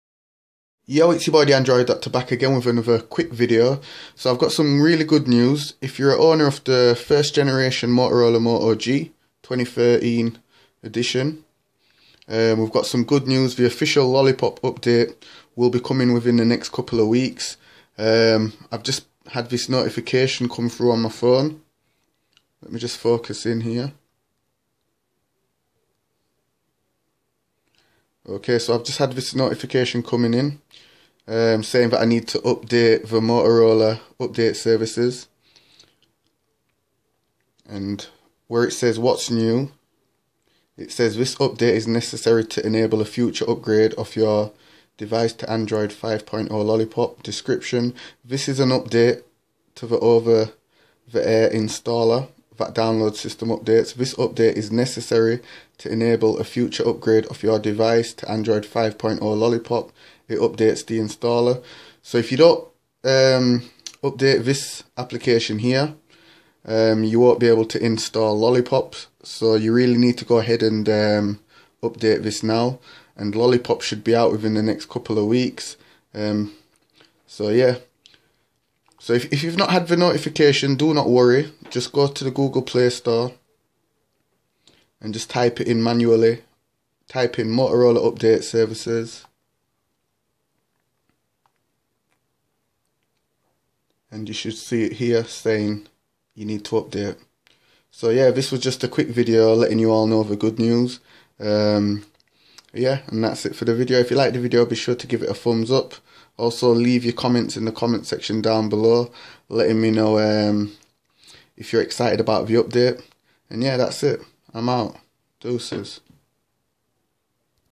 Moto G 1st Gen 2013 PREP FOR ANDROID 5.0 LOLLIPOP OFFICIAL OTA
This is just a short video explaining how to prepare your moto g 2013 model for the official android 5.0 ota lollipop update